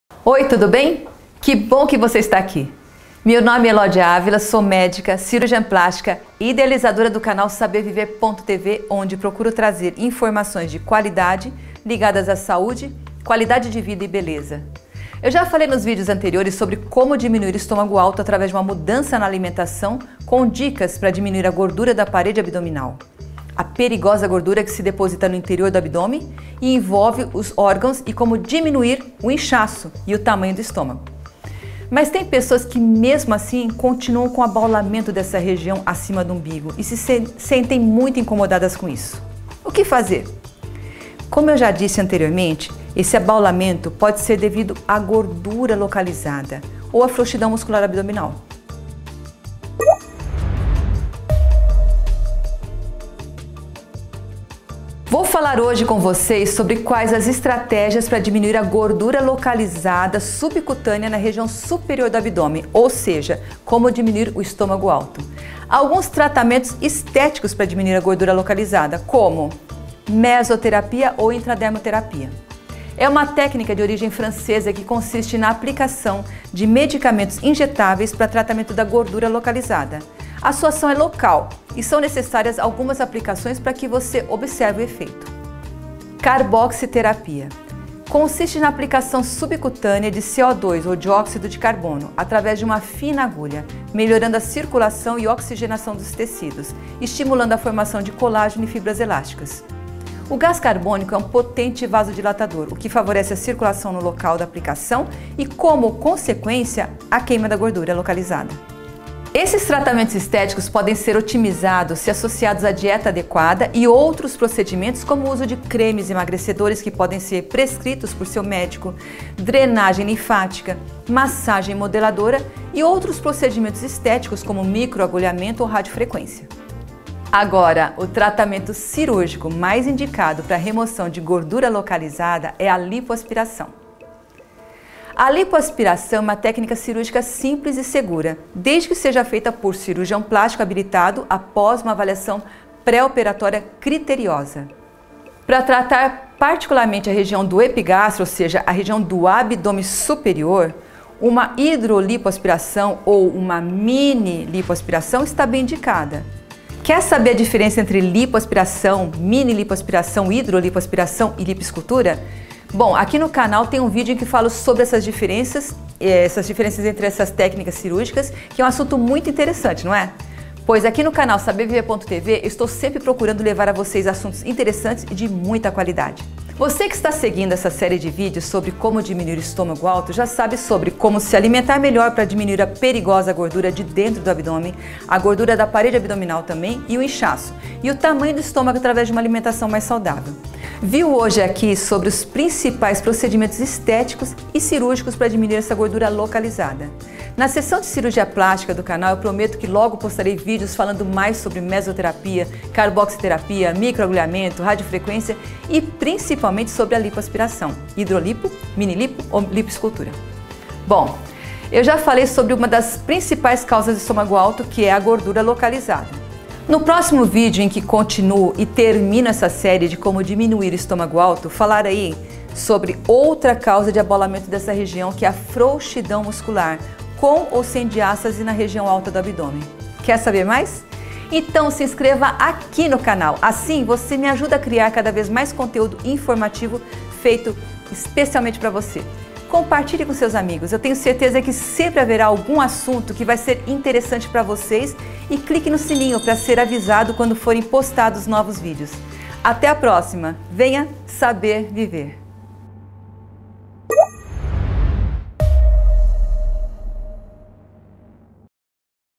Como diminuir o estômago alto com o saúde?
Sou a Dra Elodia Avila Cirurgiã plástica.

(Episodio 03)

Muitas pacientes me perguntam como diminuir o estomago alto então resolvi gravar o vídeo para ajudar no Bem Estar, beleza, e saúde de todas vocês.

Me conte nos comentários se você também tem estomago alto.